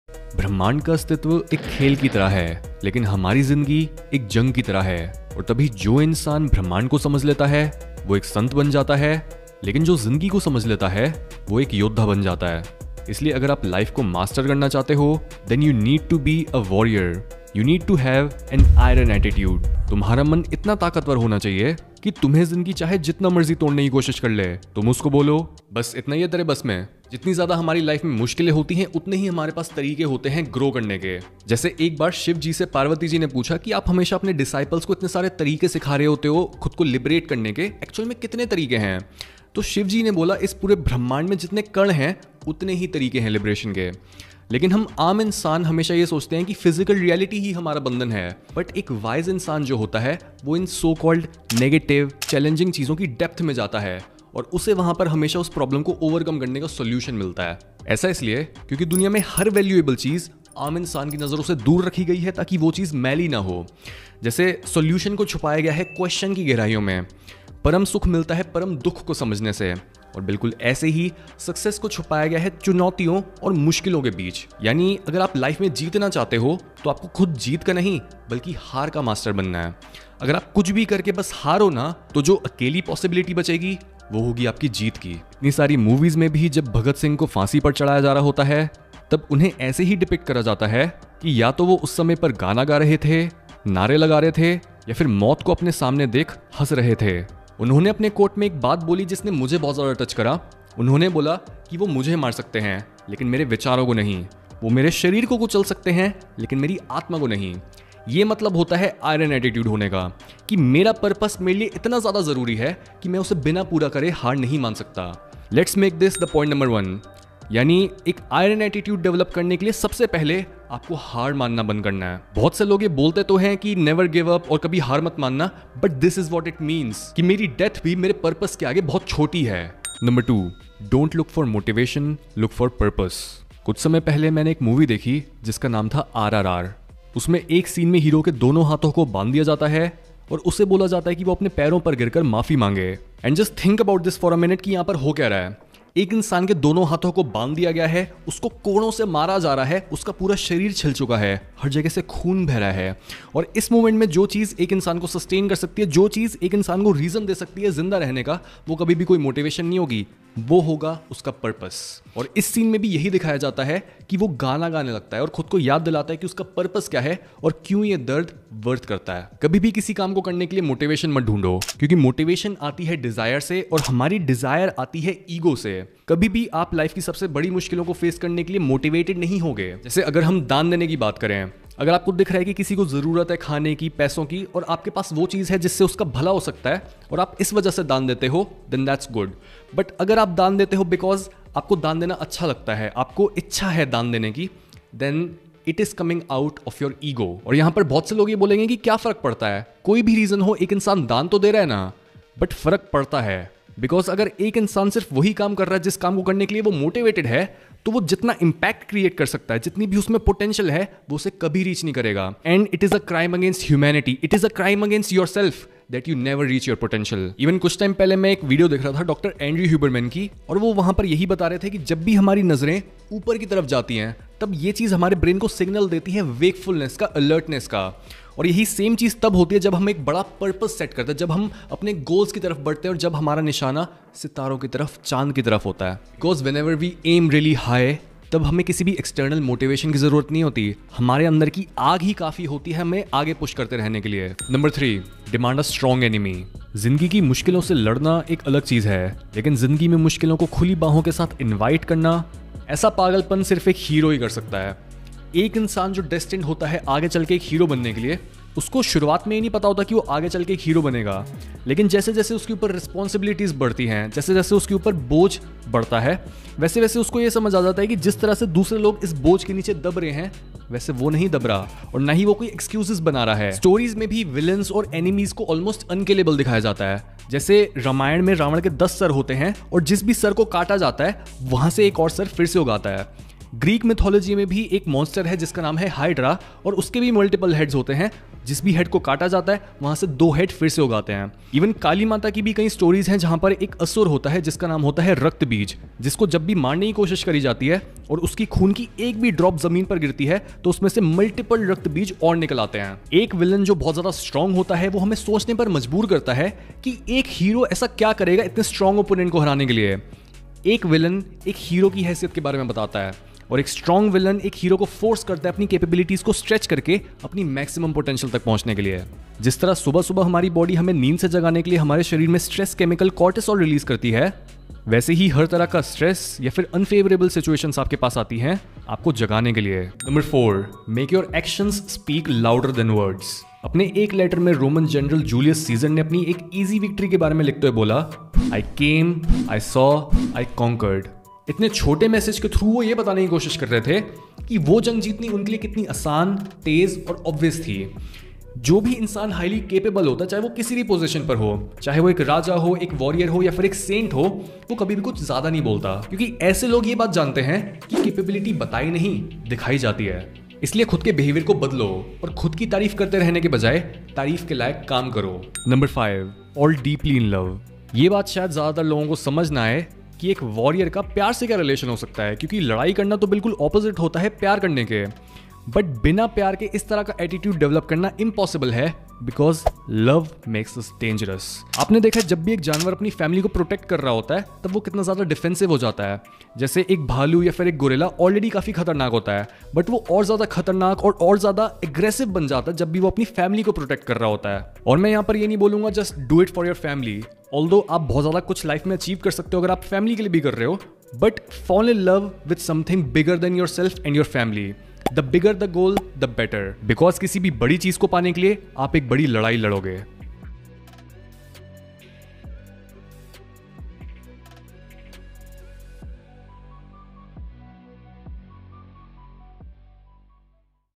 IRON ATTITUDE - The Only Way To Succeed In Life
Life is war and to win this war, you need an iron attitude.

0:00 Why you need an Iron-Attitude
0:48 Good resides in bad
1:25 Bhagat Singh's Iron Attitude
1:47 Don't give up
2:09 Motivation is useless
2:59 Selfless vs Selfish donation
4:04 Demand a strong enemy
4:31 Why an enemy is hard to kill
5:02 Stress wakes us up
5:29 Do more, speak less
6:05 Love makes us dangerous